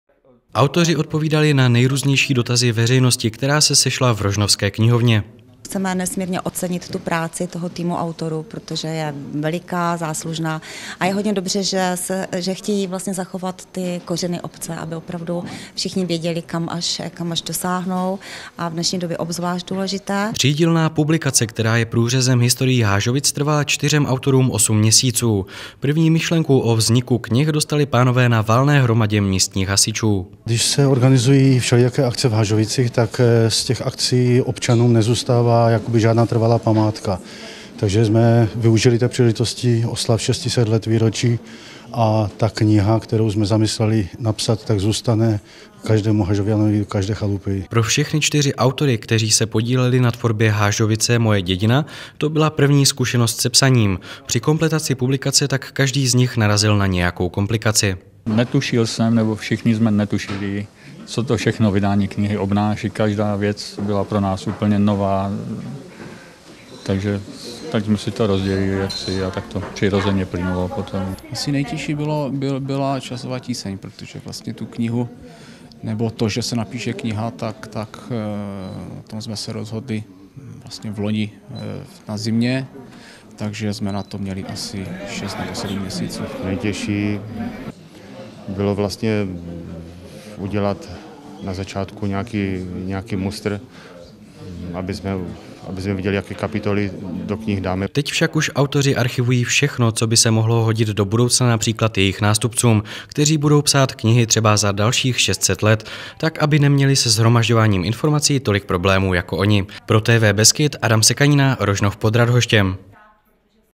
Hážovice moje dědina
V městské knihovně v Rožnově pod Radhoštěm se konala beseda s názvem Hážovice -- moje dědina. Tam vystoupila skupina autorů stejnojmenné publikace knih k příležitosti 600 let od první zmínky o Hážovicích.